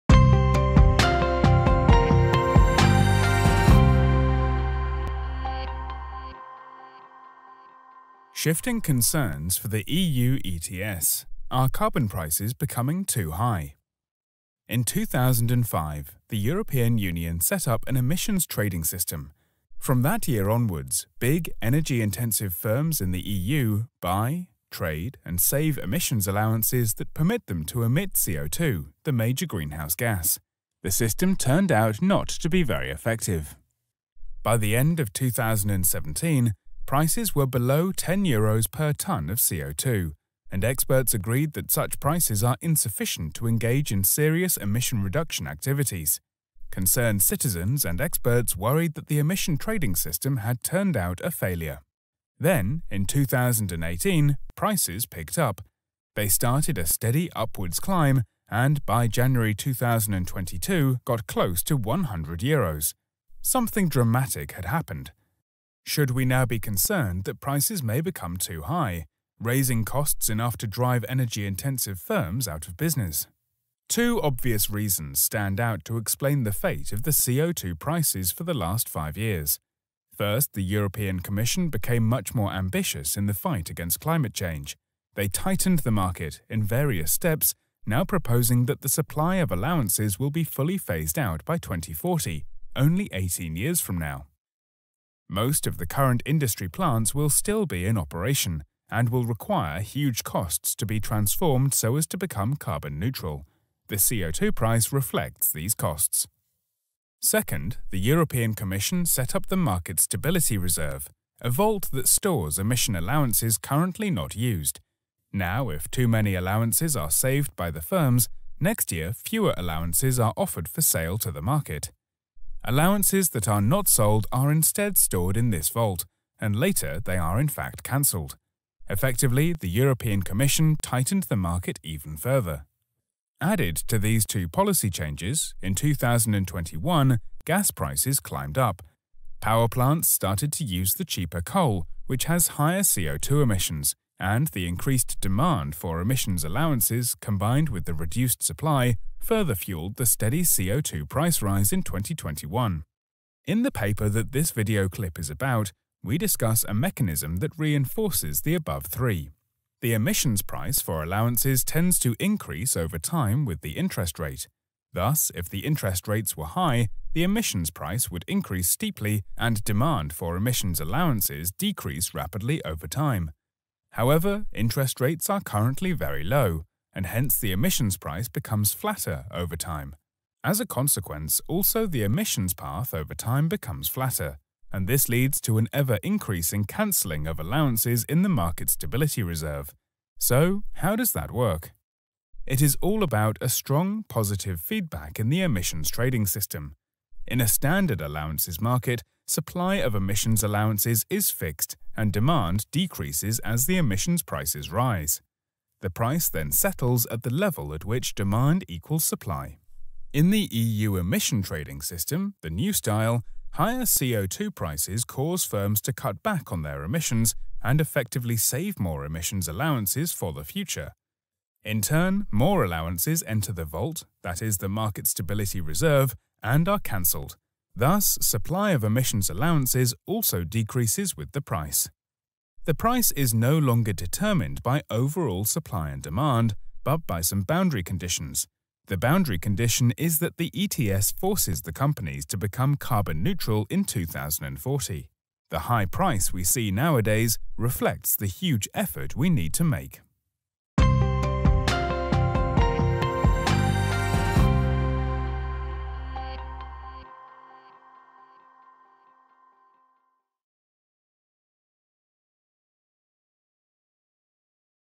Shifting concerns for the EU ETS: are carbon prices becoming too high?
Carbon prices in the EU have risen to close to 100 euro per ton CO2. These prices are much higher than elsewhere in the world. This knowledge clip explains why that is. It is based on the article 'Shifting concerns for the EU ETS: are carbon prices becoming too high?' written by Reyer Gerlagh, Roweno Heijmans and Knut Einar Rosendahl.